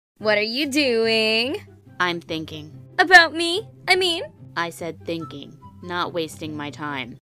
What Is Wednesday Thinking About? | Wednesday Comic Dub
In this Wednesday Comic Dub, Enid finds out what Wednesday is thinking about. But What is Wednesday thinking about? Find out yourself in this Comedical Comic Dub

🎙️People Involved 🎙️

BlueKittenCosplay as Wednesday